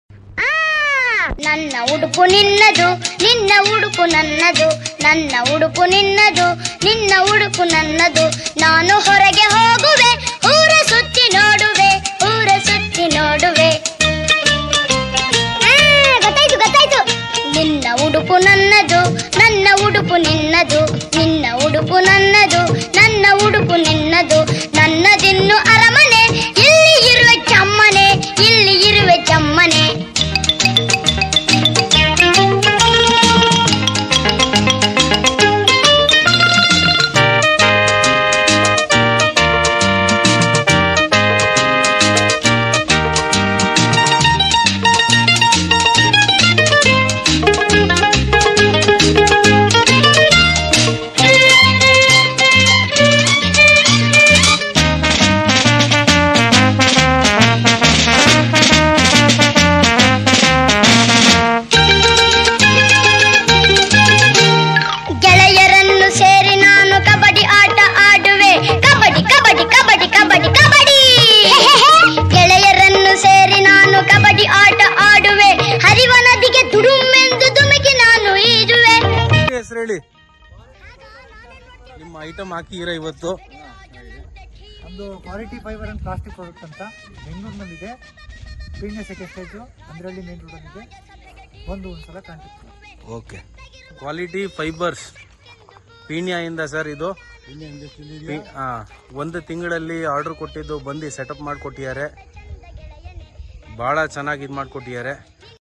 "Colorful Playground Fun: New Multiplay Station Setup!" for school
"Transform your outdoor space with our vibrant and durable multiplay station! Perfect for schools, resorts, preschools, and parks, this playground set is designed to provide endless fun and adventure for kids. With its colorful slides, tunnels, and bridges, it's built for safety and excitement.

**Quality Fiber And Plastic Products** – Premier Playground Equipment Supplier & Outdoor Gym Equipment Manufacturer since 2001.

📍Byraveshwara Industrial Estate, Peenya, Bengaluru